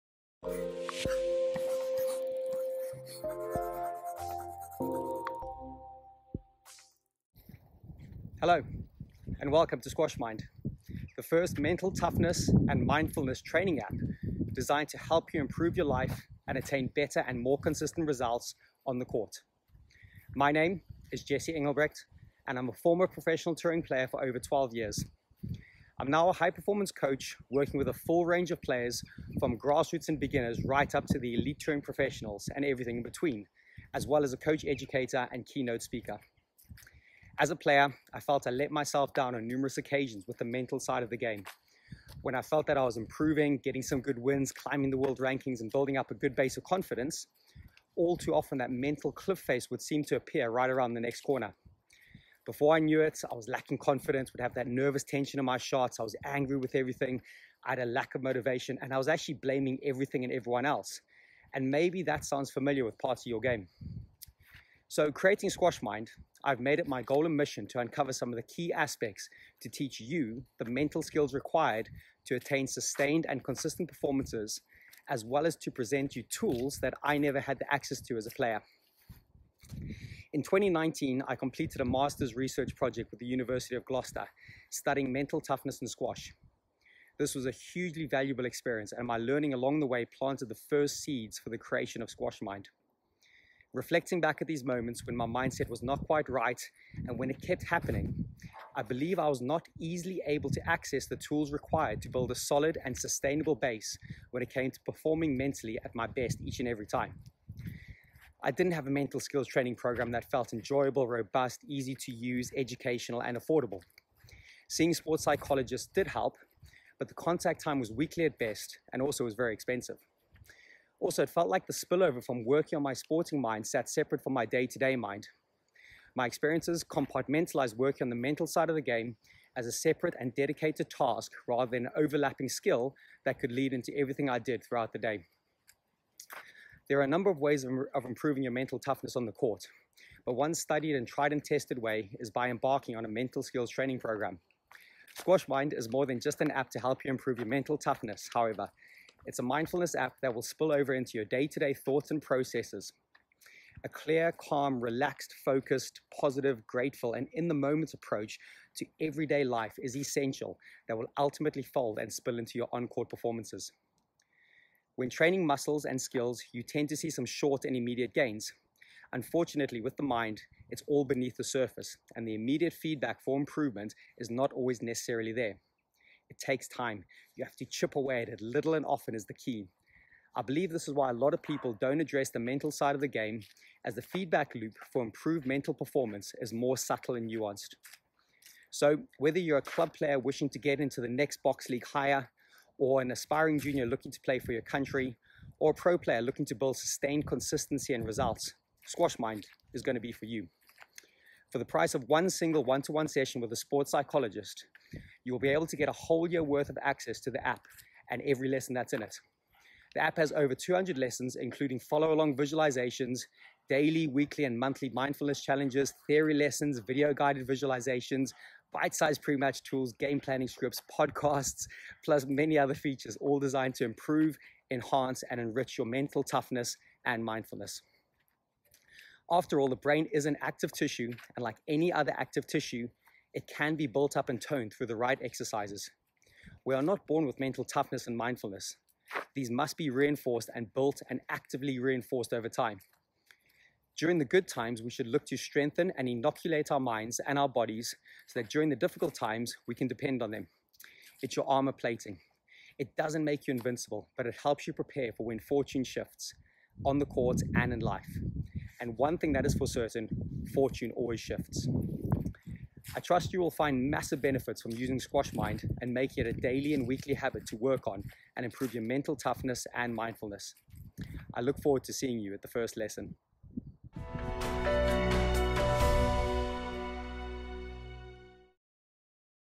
Welcome to SquashMind
All you need to know about SquashMind and what you can expect from the app and the training